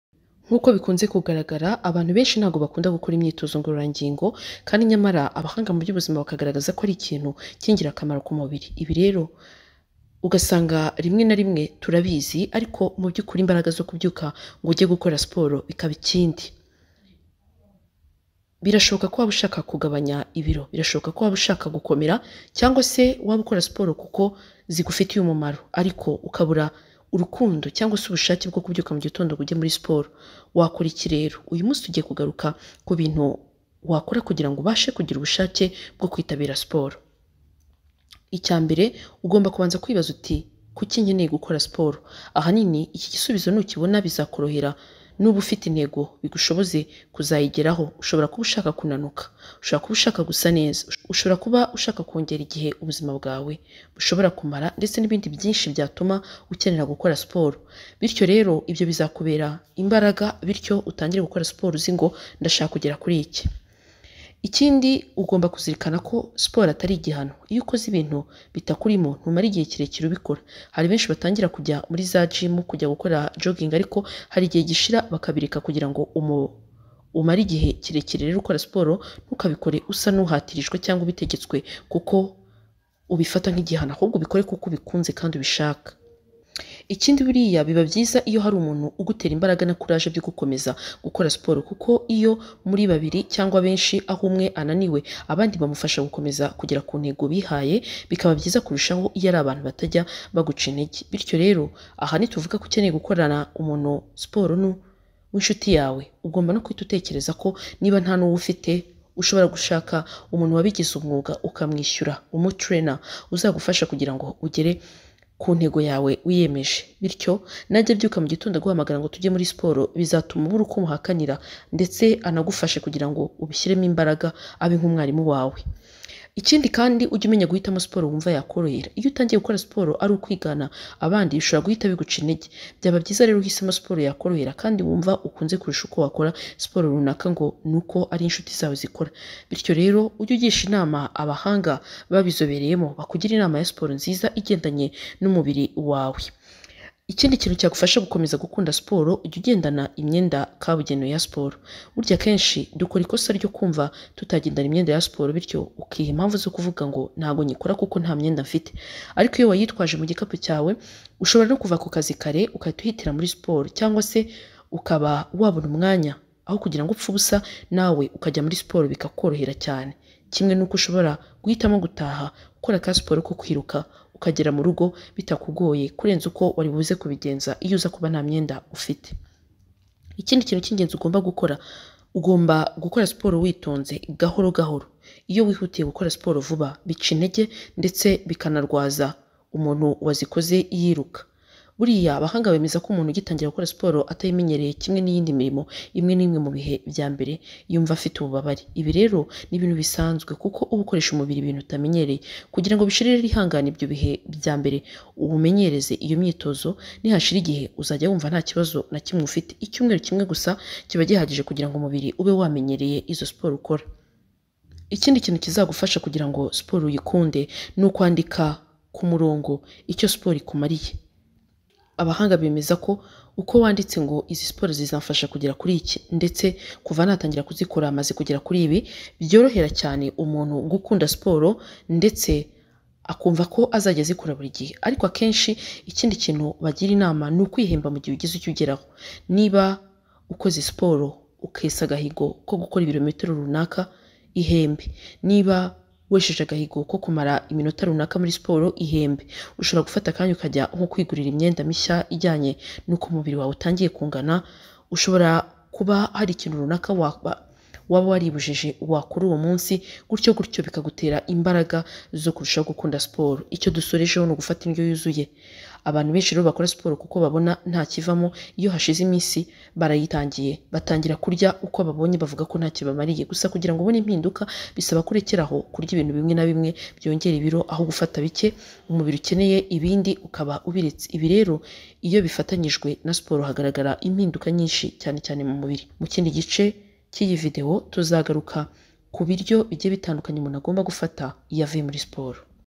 IBINTU BYATUMA UKUNDA SPORT/ NAKORA IKI NGO MPOZEHO/ SPORT IMARIYE IKI
Ikiganiro hamwe na Delphine Umuhoza
uge ukurikira ibiganiro by'Ubwiza hano